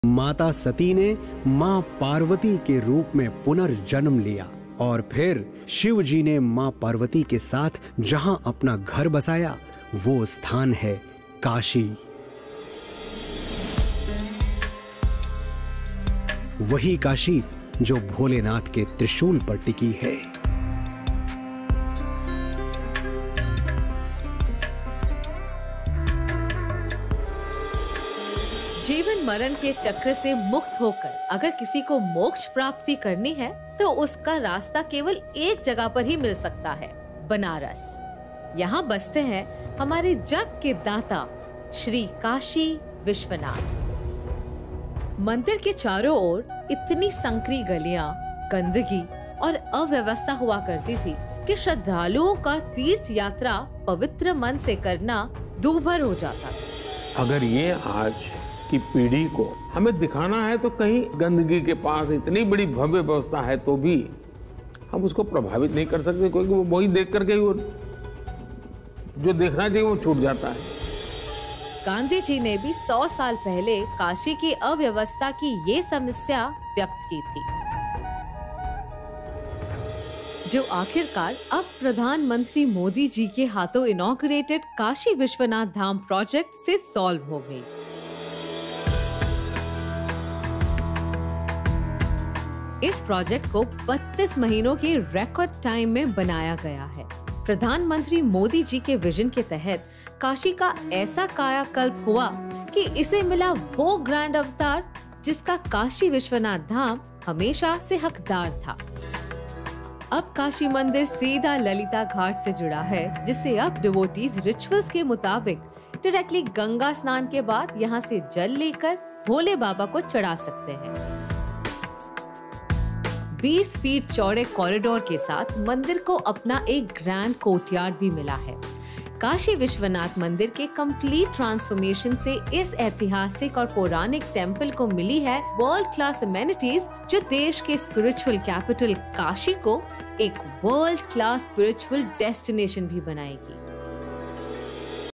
Kashi Vishwanath Dham | A tribute to the abode of Lord Shiva | PM Modi
Kashi Vishwanath Dham often regarded as the abode of Lord Shiva and Mata Parvati was often susceptible to unclean surroundings creating nuisance for the devotees. The Kashi Vishwanath Dham project has changed perceptions and has sought to promote clean surroundings for devotees enabling a unique spiritual experience.